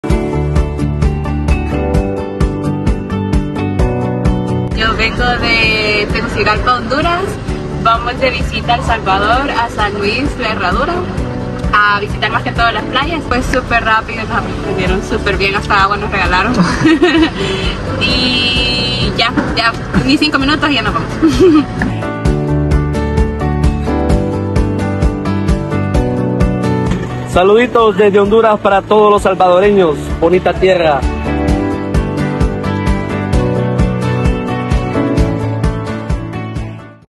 Frontera El Amatillo
El Salvador, un destino ideal que eligen visitar centroamericanos y turistas de todo el mundo.

Hondureña nos compartió su experiencia realizando su control migratorio por frontera El Amatillo.